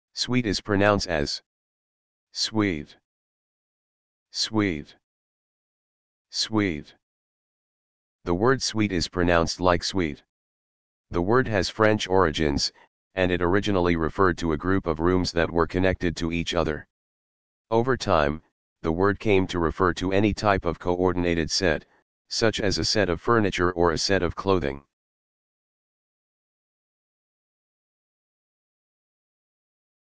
How To Pronounce Suite
The word "suite" is pronounced like "sweet." The word has French origins, and it originally referred to a group of rooms that were connected to each other. Over time, the word came to refer to any type of coordinated set, such as a set of furniture or a set of clothing.